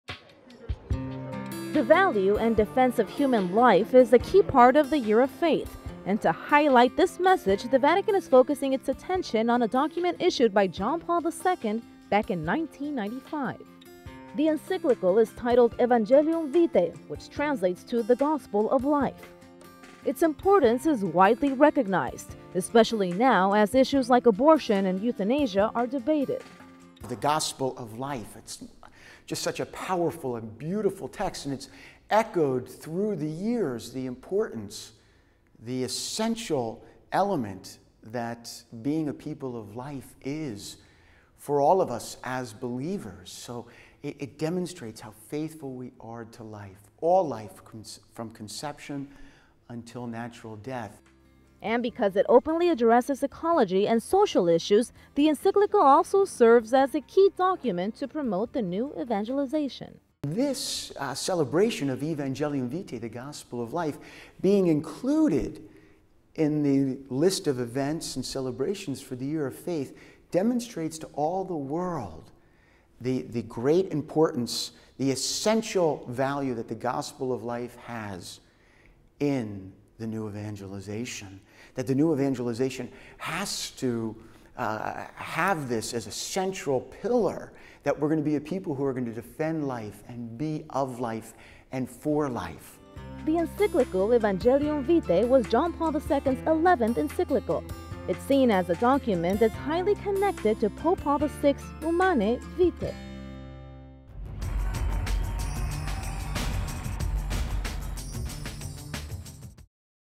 Evangelium Vitae: the 'Gospel of Life'
The value and defense of human life is a key part of the Year of Faith. To highlight this message, the Vatican is focusing its attention on a document issued by Pope John Paul II back in 1995.

The encyclical is titled 'Evangelium Vitae,' which translates to 'The Gospel of Life.'  Its importance is widely recognized, especially now, issues like abortion and euthanasia are debated. .